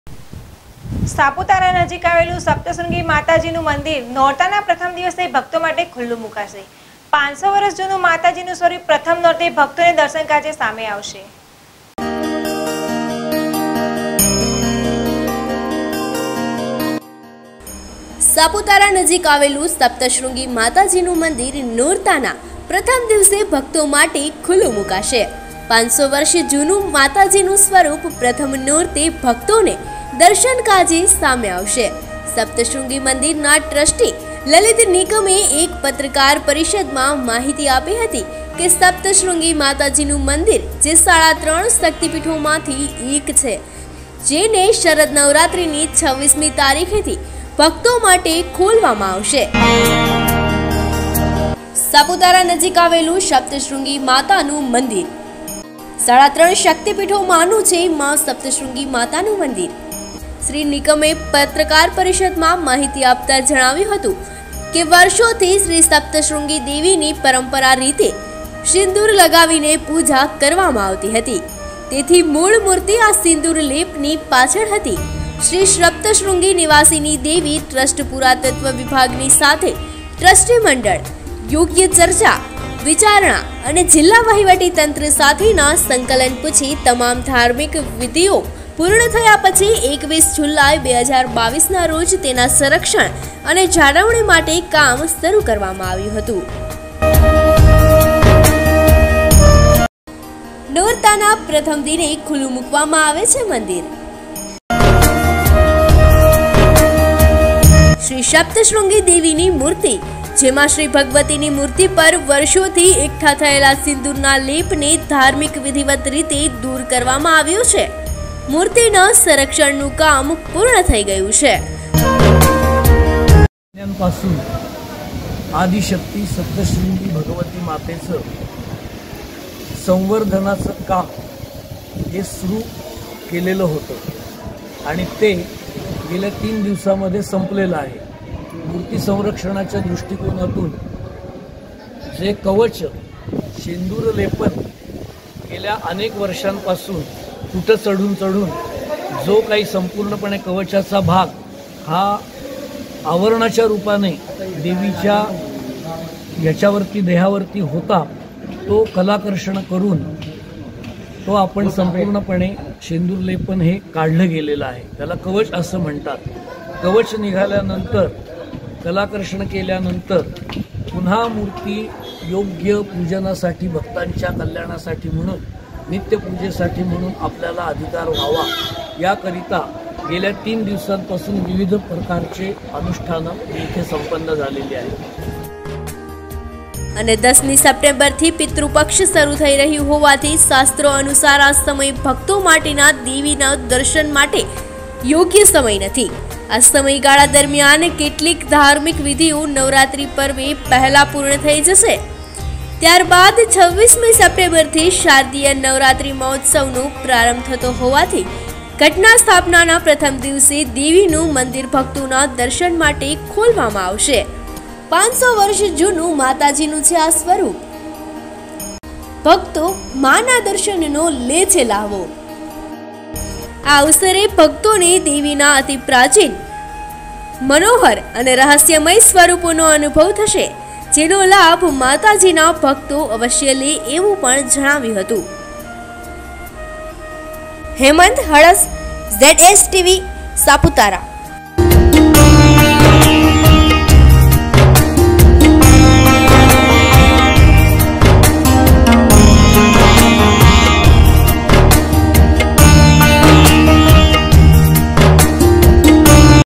સાપુતારા નજીક આવેલું સપ્તશૃંગી માતાજી નું મંદિર મુકાશે 500 વર્ષ જૂનું માતાજીનું સ્વરૂપ પ્રથમ
સાપુતારા નજીક આવેલું સપ્તશૃંગી માતાજી નું મંદિર નોરતાના પ્રથમ દિવસે ભક્તો માટે ખુલ્લુ મુકાશે 500 વર્ષ જૂનું માતાજીનું સ્વરૂપ પ્રથમ નોરતે ભક્તોને દર્શન કાજે સામે આવશે